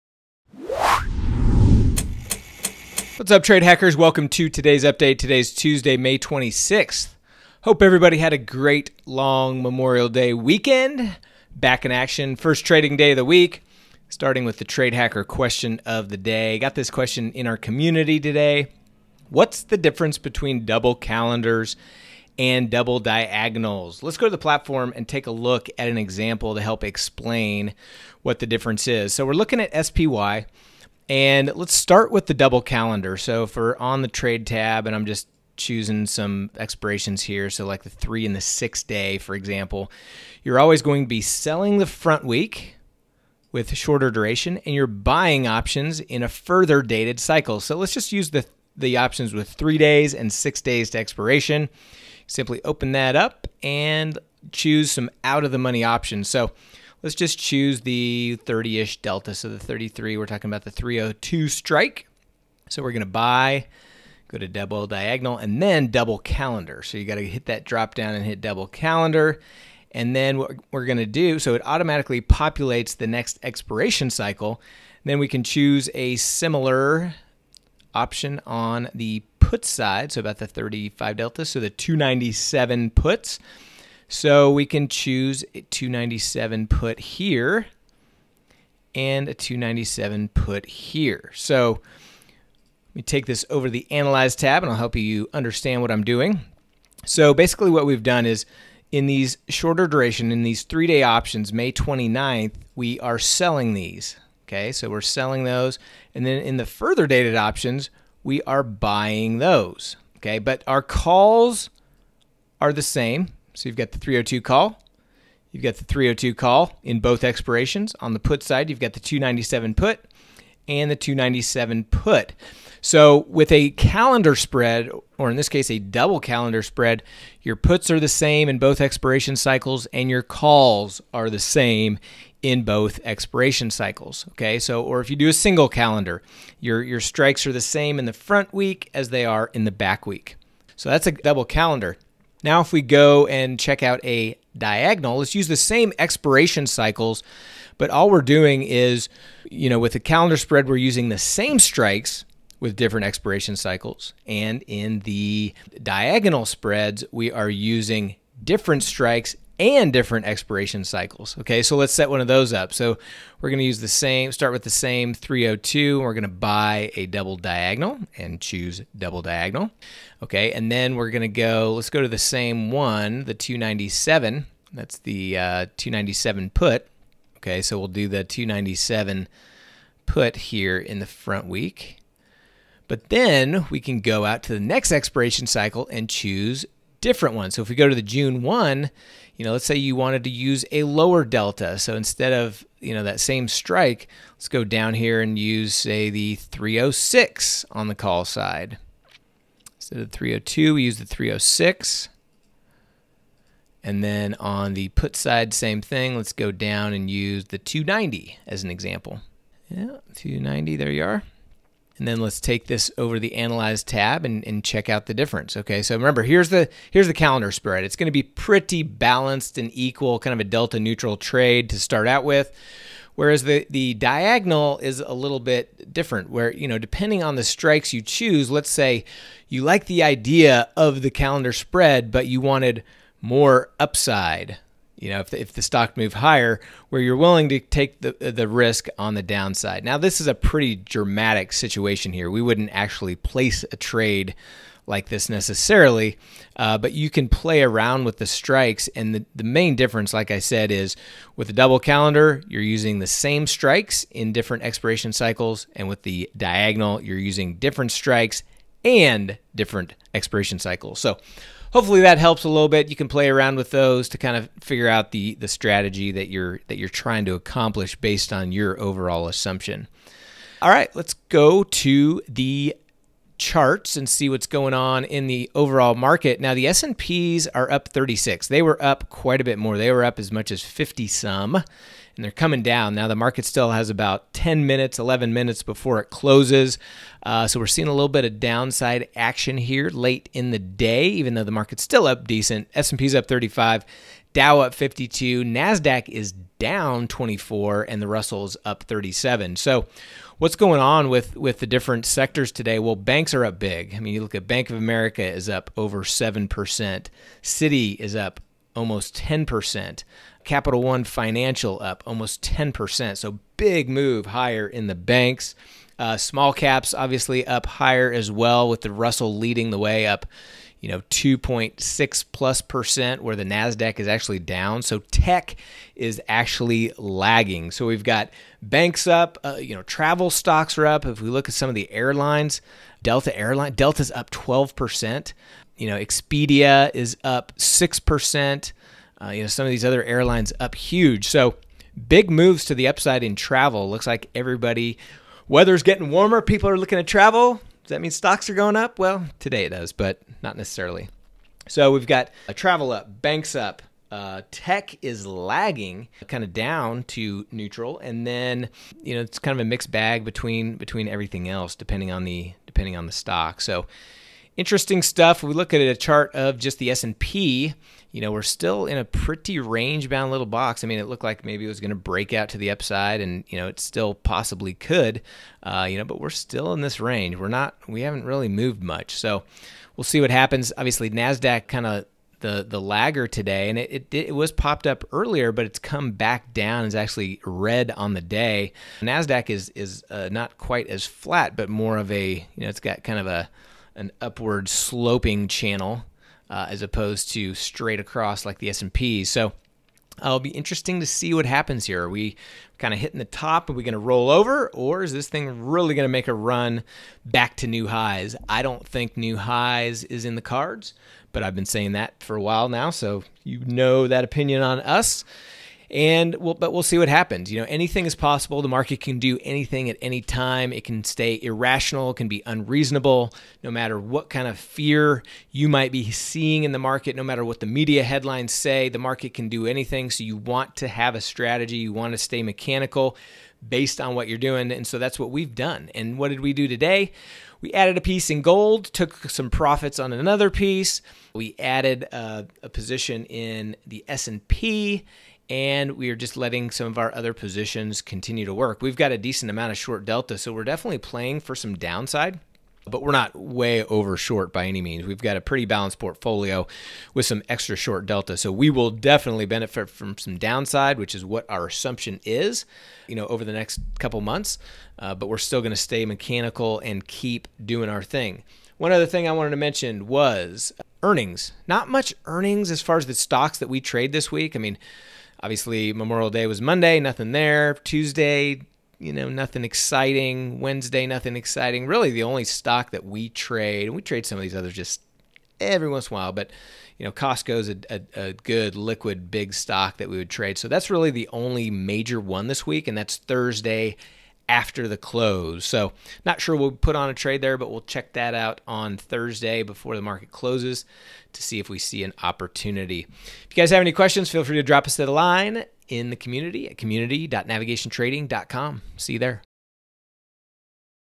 Banks and Travel are UP [TradeHacker Update]
Here's what we'll cover in today's update:
• Banks and Travel are UP
• Tech is DOWN
• What's the difference between a Double Calendar & Double Diagonal?

Happy Trading!

YouTube.com/navigationtrading
Facebook.com/navigationtrading